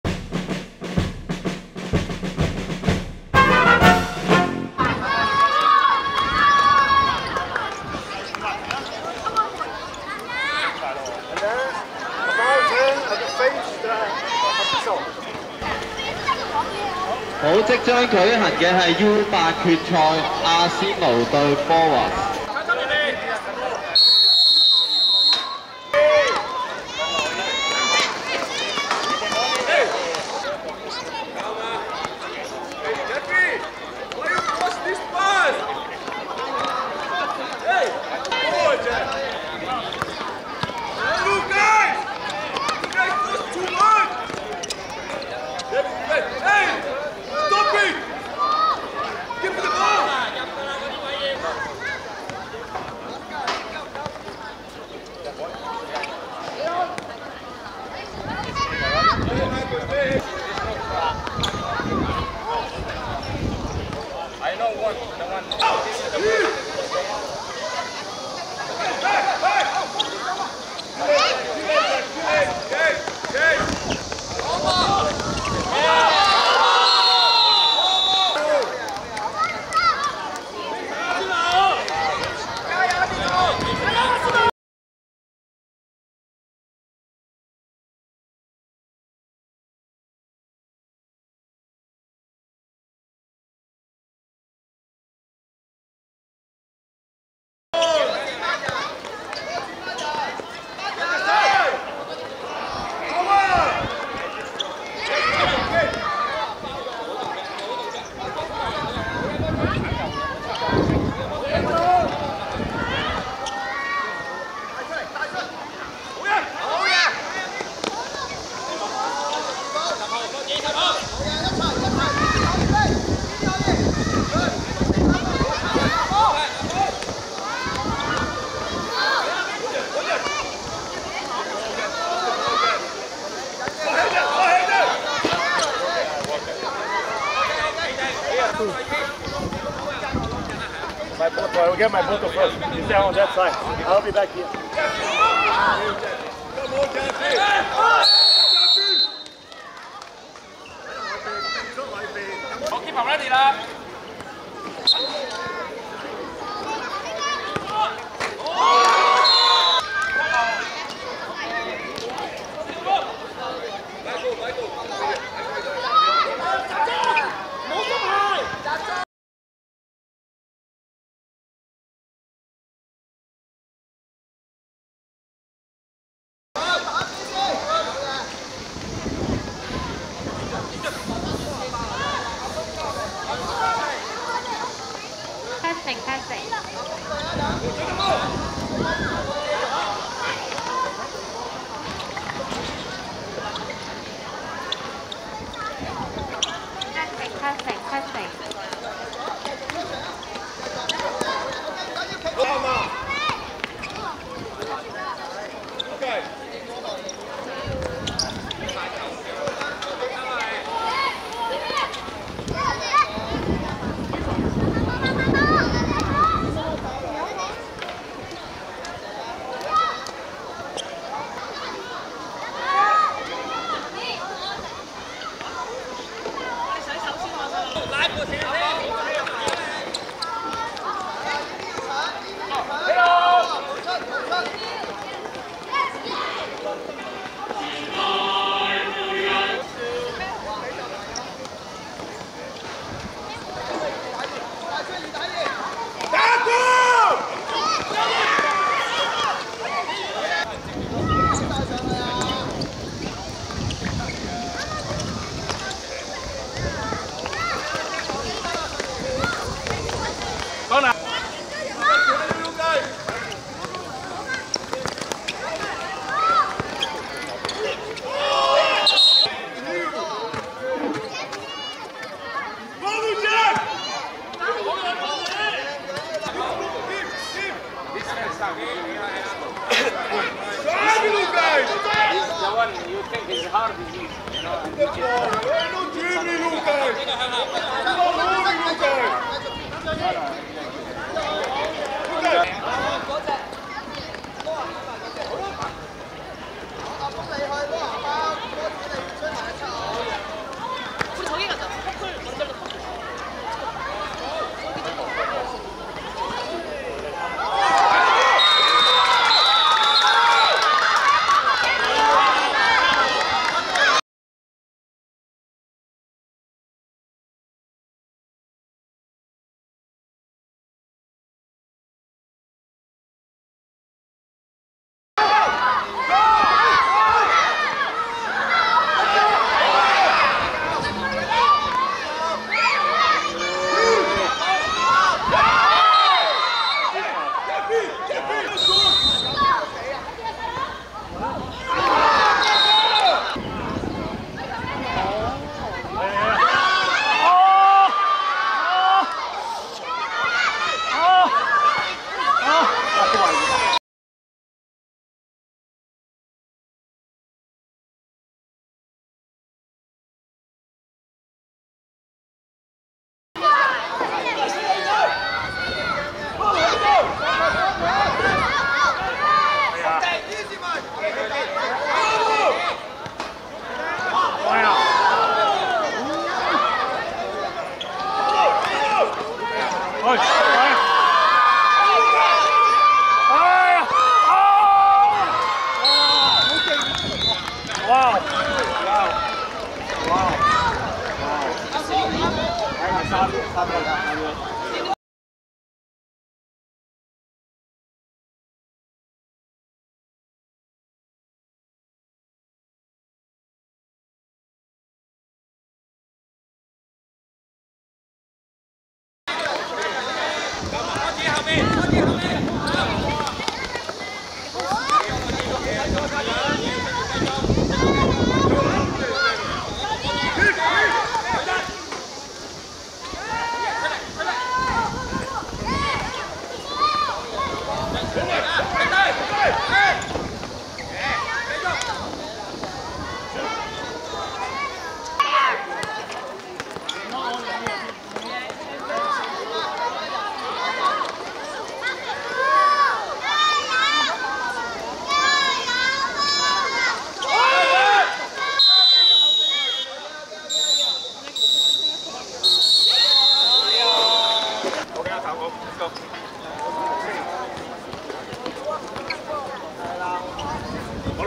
Arsenal vs Boas (2024.7.1.We Sports回歸盃5人足球賽~U8決賽)精華
2024.7.1.厦村足球塲 15:25
Arsenal(綠) 3 : 1 Boas(橙)